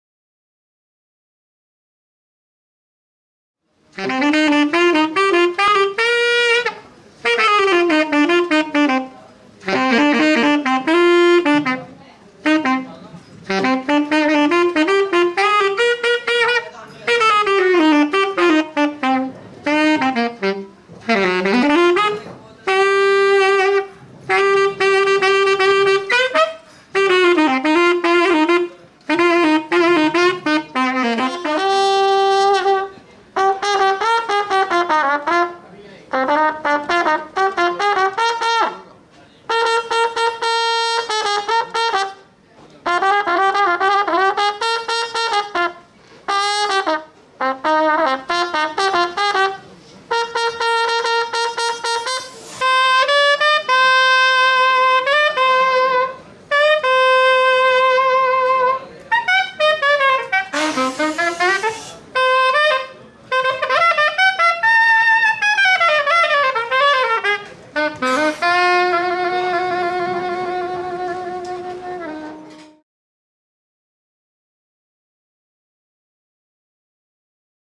Johnny Fernandes - saxophone and trumpet demo
Goa's veteran saxophonist also plays trumpet and he is an amazing showman!